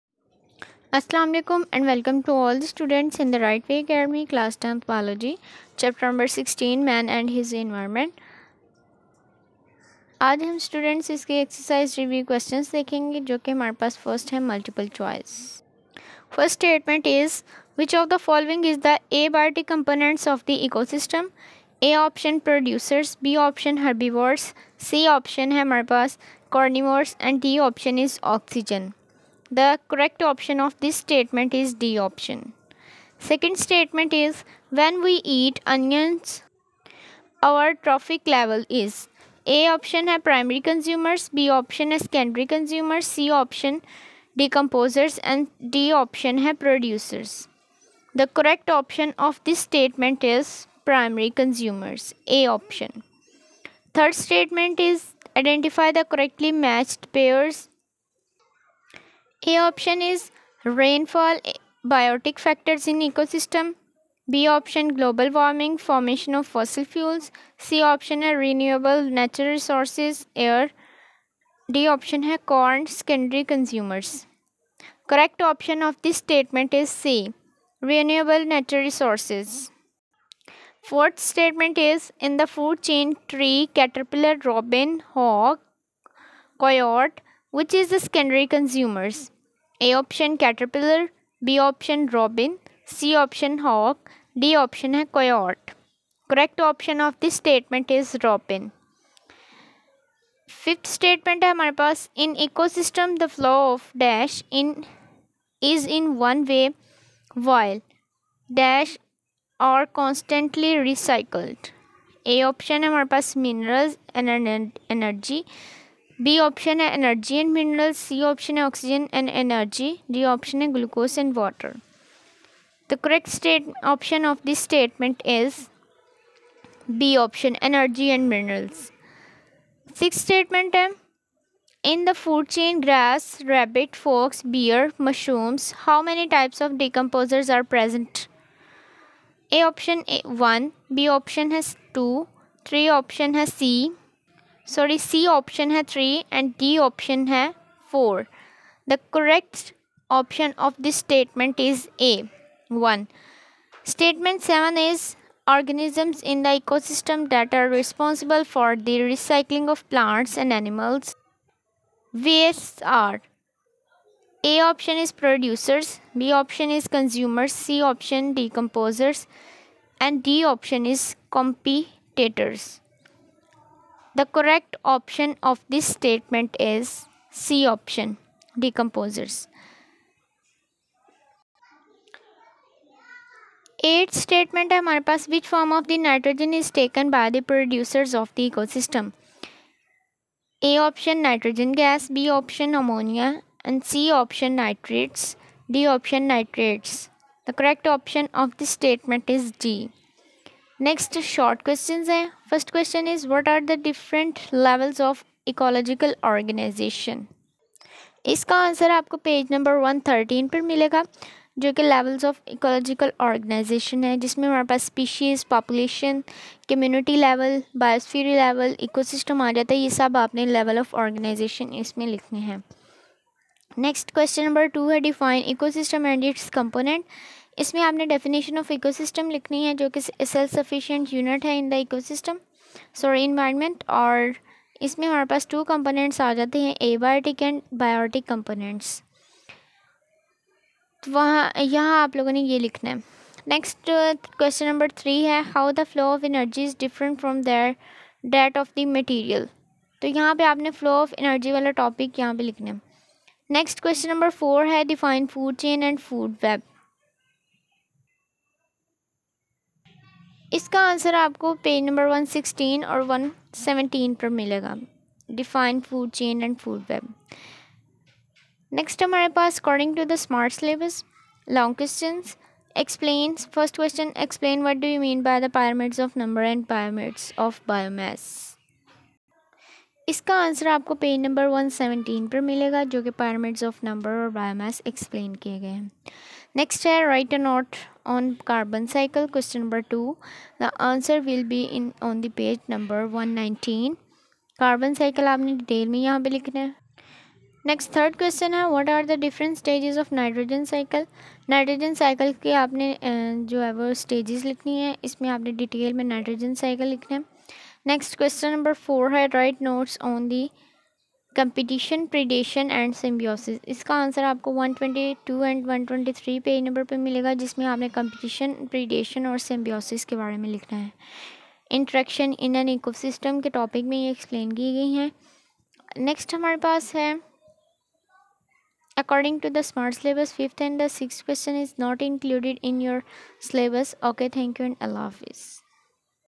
Class 10 biology | Exercise M.C.Qs short and long questions | chapter 16 | Man and his Environment.
In this vedio, the complete exercise solution of m.c.qs , short and long questions of this chapter .
Man and his Environment.
Class 10 biology followed by Punjab text book boards according to smart syllabus.